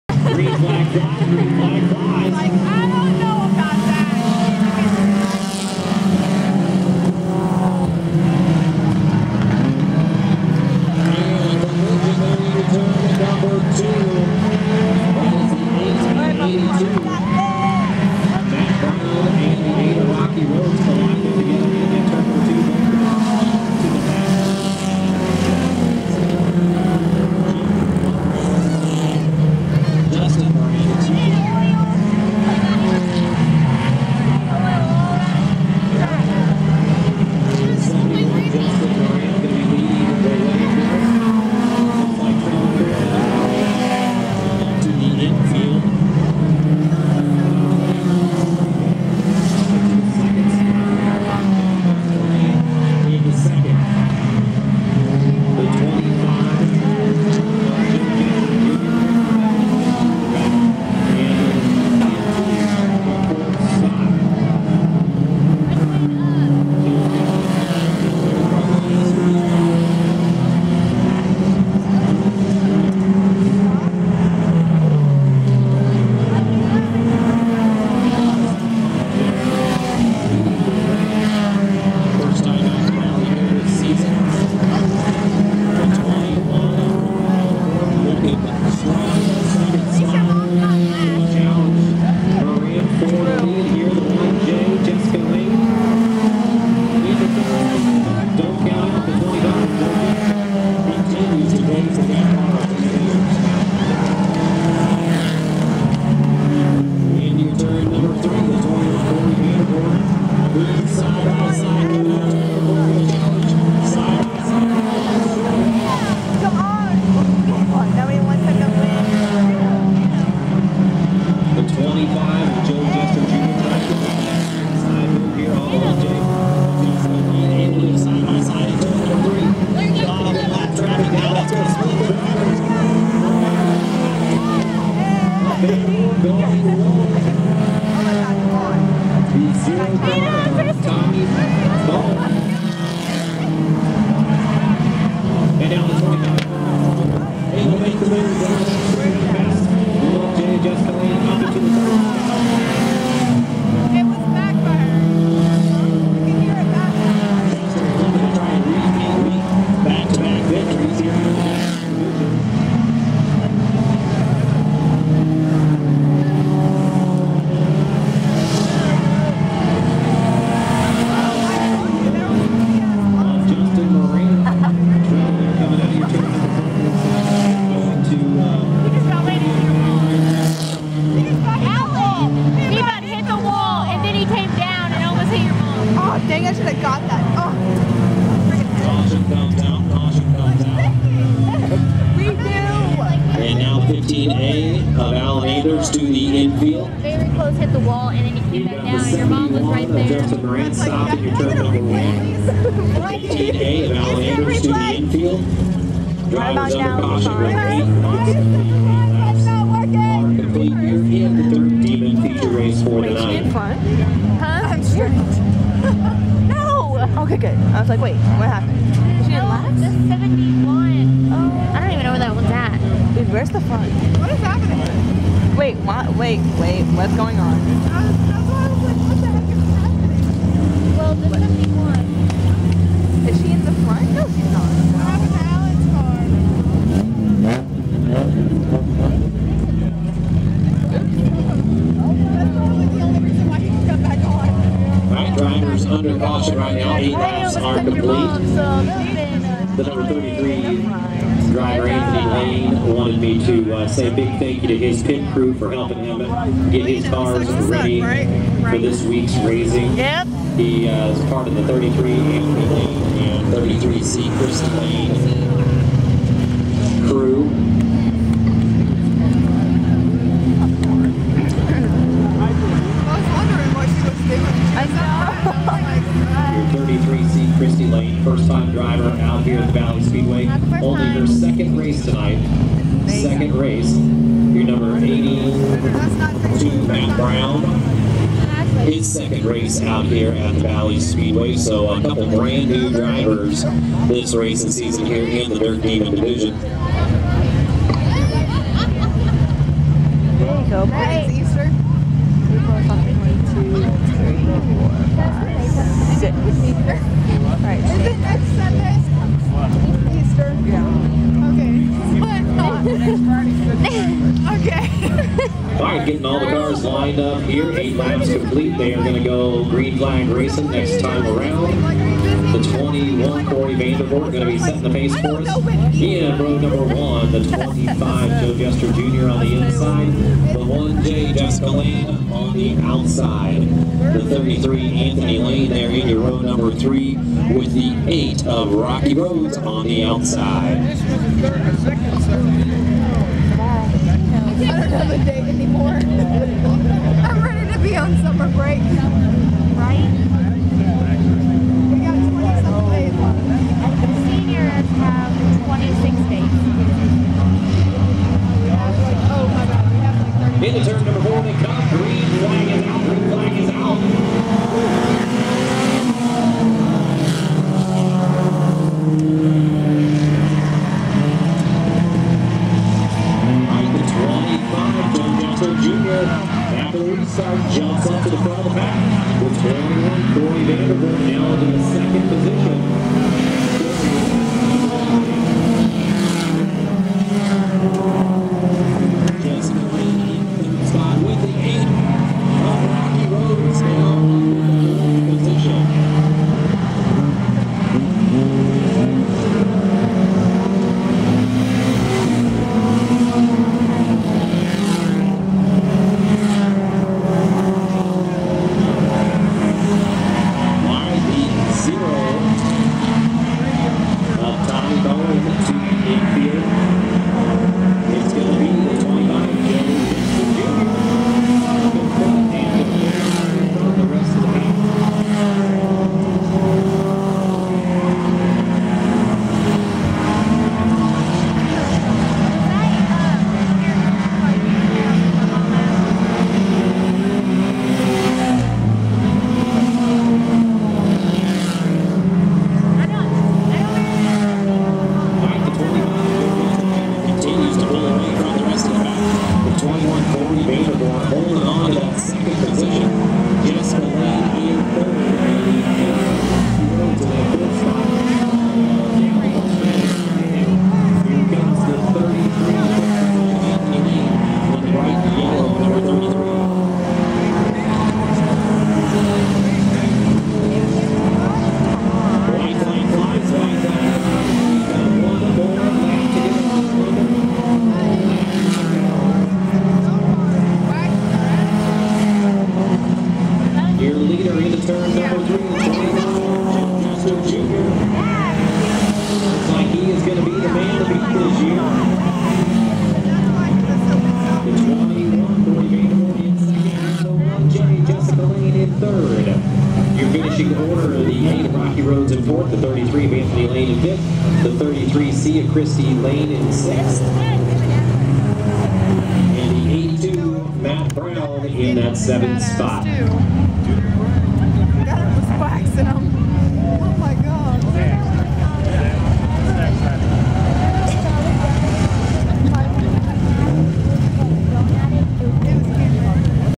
Dirt Demon class at Valley Speedway /grian valley mo 4-12-14
Dirt Demon class at Valley Speedway /grain valley mo.

wife is in the purple 33 and I am in the yellow mini late model looking